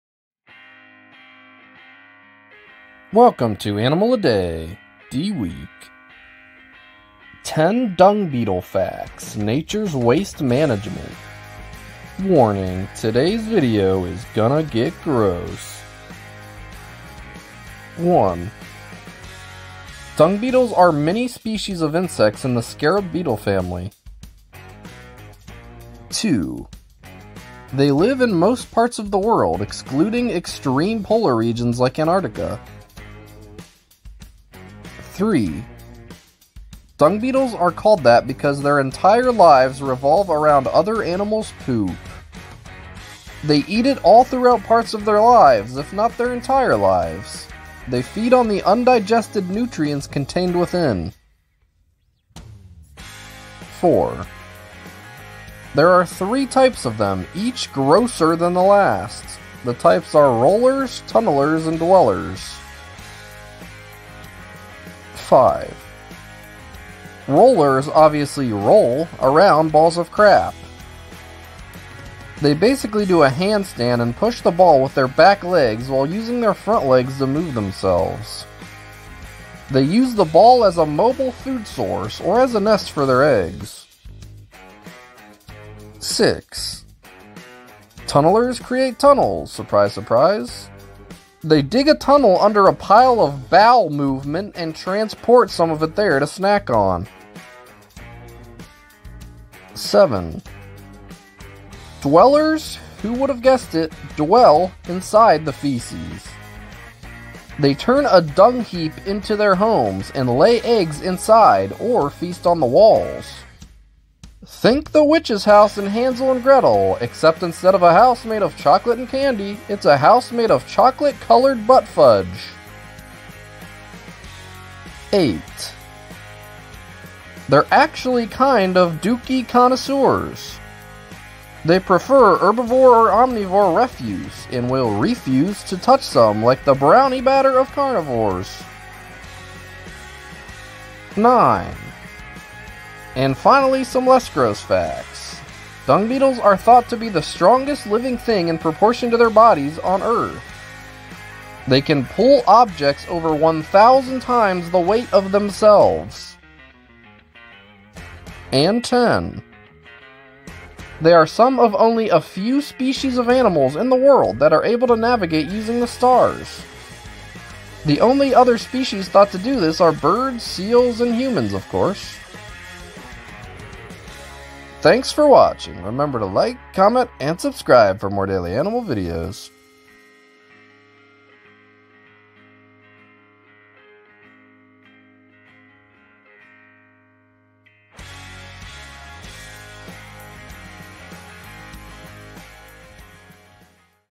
10 Dung Beetle Facts - Nature's Waste Management - Animal a Day D Week
Welcome to Animal a Day's D Week. Every day this week will have an animal that starts with the letter D. Today we learn about the insects who love to live in, eat, play with, and party in heaping piles of animal excrement, the Dung Beetle. (This video is pretty gross)